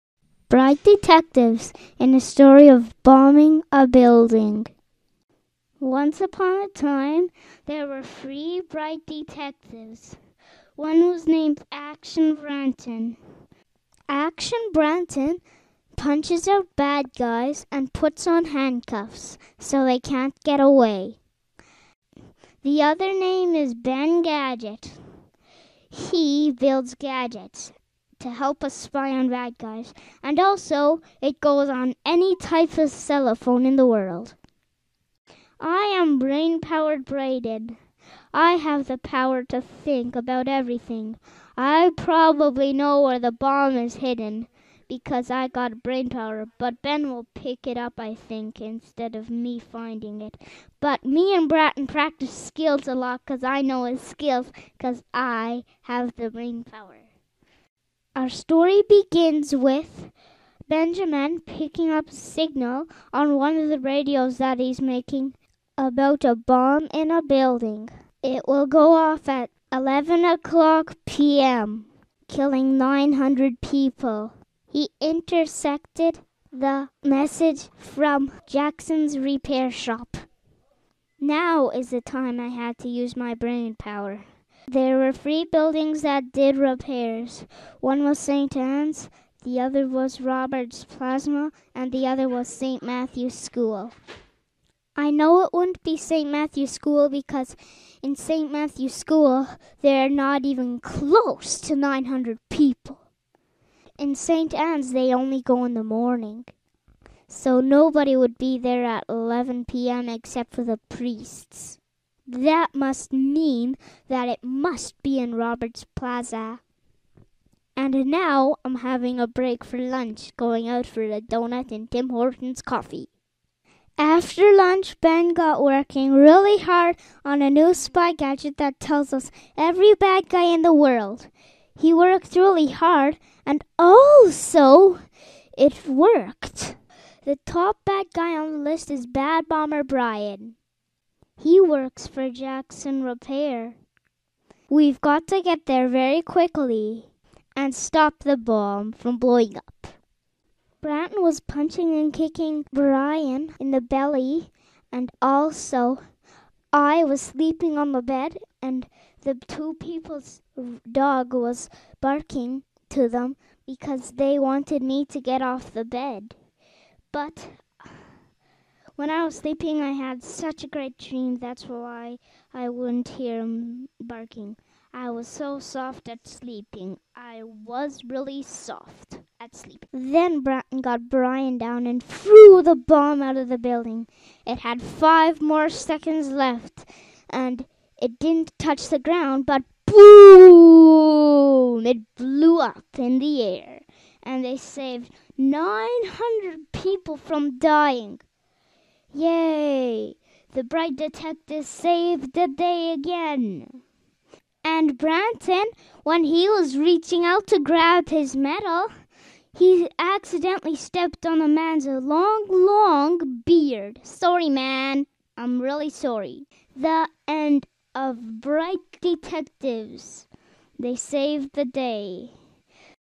Breit Detectives
This video was made by Braden in Garage Band when he was in Grade 2. He was probably 6 or 7 years old at the time. THIS is EPIC